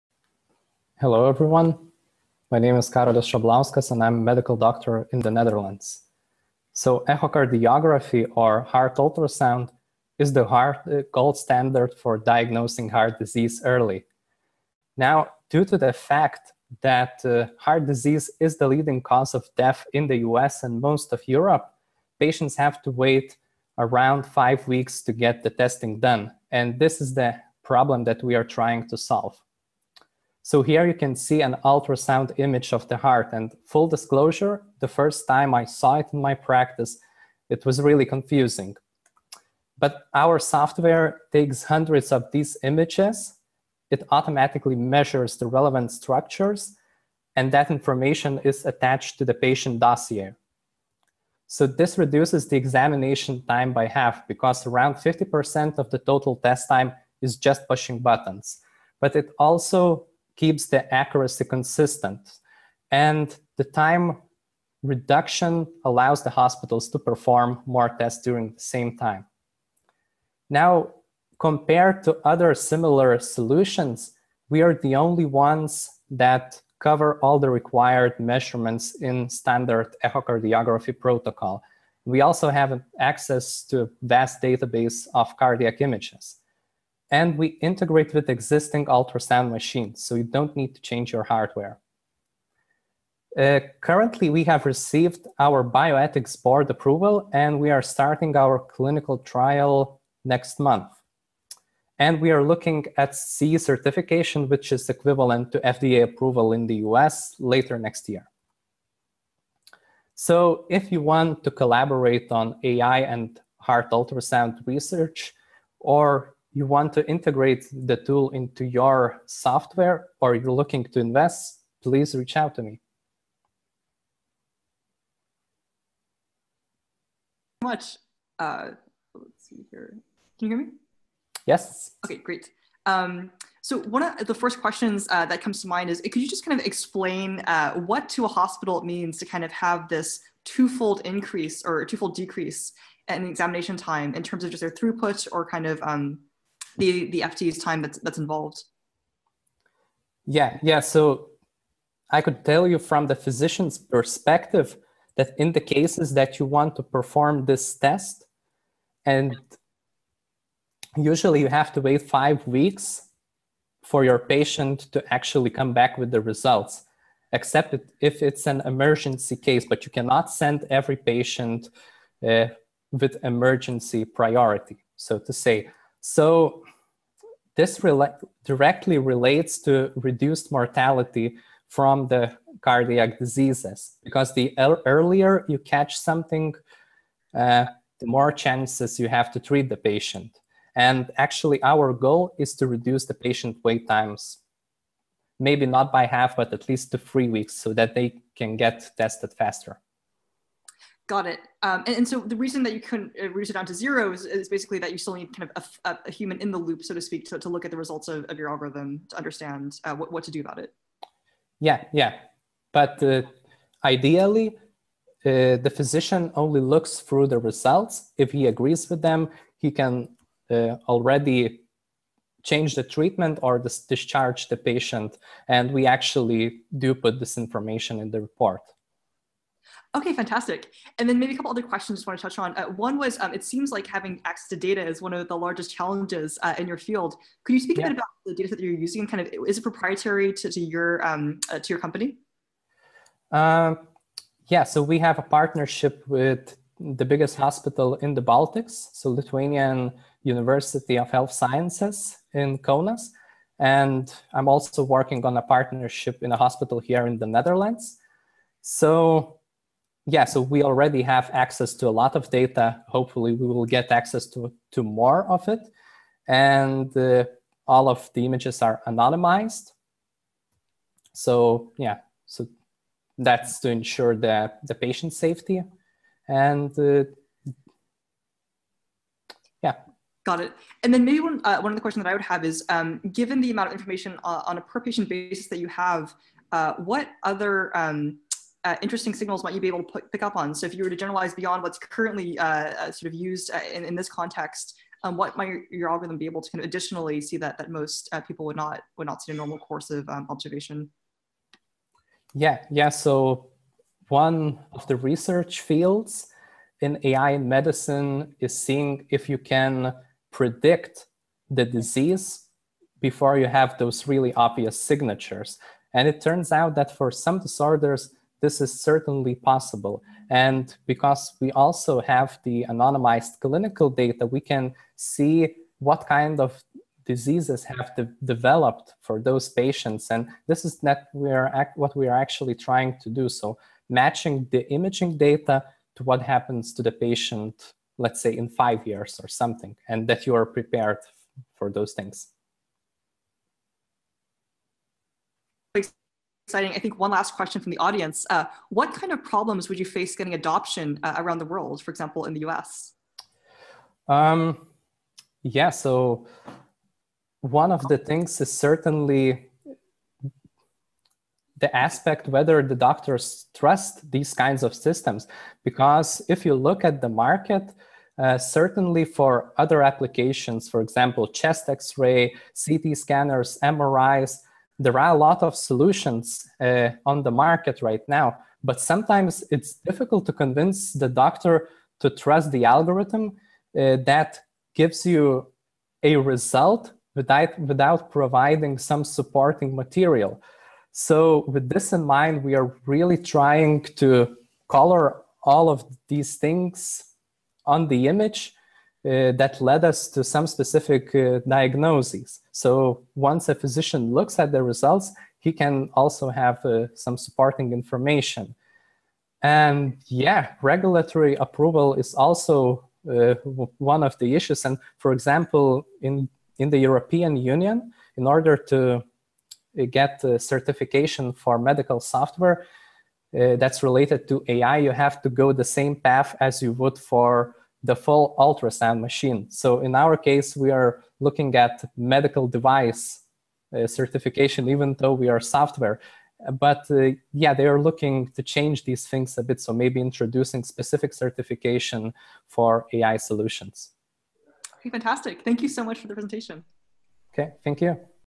Karolis Šablauskas presents Ligence to Laura Deming — Pioneer Livestream Day 2
Karolis Šablauskas presents Ligence automated heart ultrasound analysis. Karolis is interviewed by Longevity Fund founder Laura Deming.